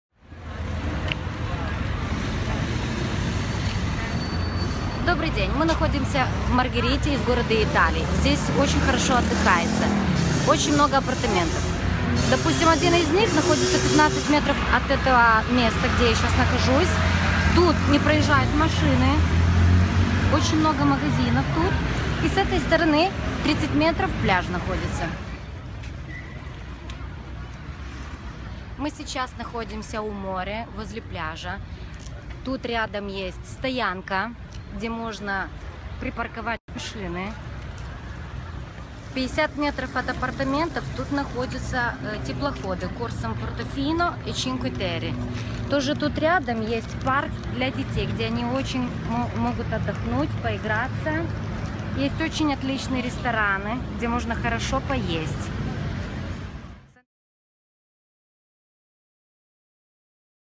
position of apartment via dell'Arco: in russian (в аренду в Санта-Маргерита-Лигуре)
Квартира в 35 метрах от пляжа, менее чем в ста метрах от центра города и туристических лодок находится в 50 метрах.
Видео создано с Apple,
Informations and indications about the zone of the apartment in Russian